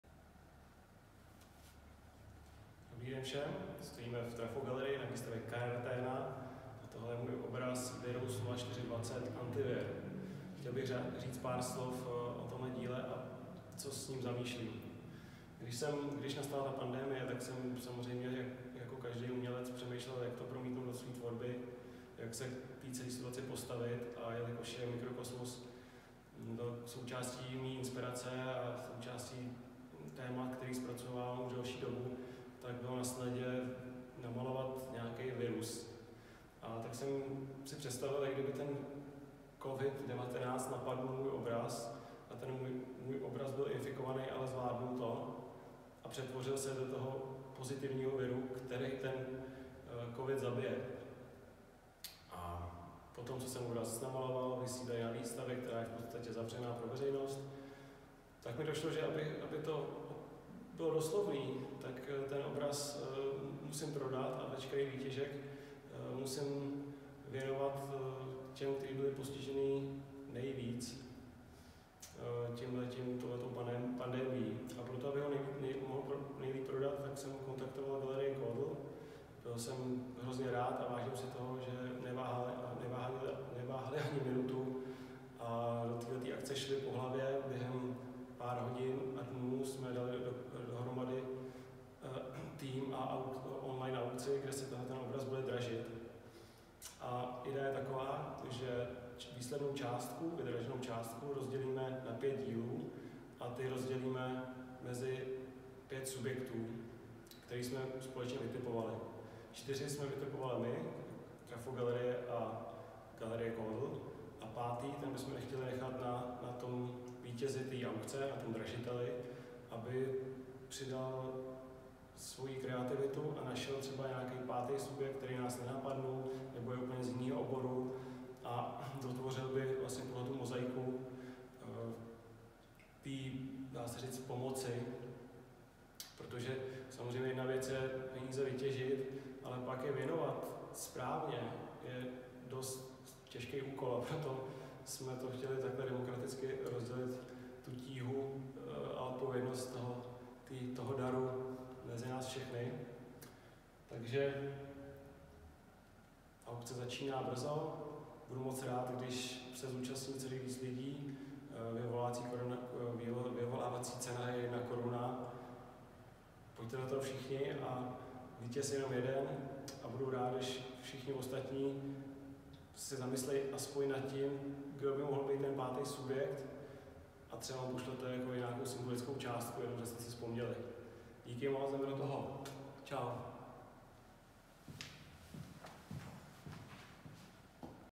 Charitativní aukce – Jan Kaláb / Virus 0420 (anticovid)
Jan Kaláb představuje své dílo Virus 0420 (anticovid), které můžete od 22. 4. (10 hod) – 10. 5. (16 hod) dražit v charitativní online aukci pořádané Galerií KODL a Trafo Gallery. Více info zde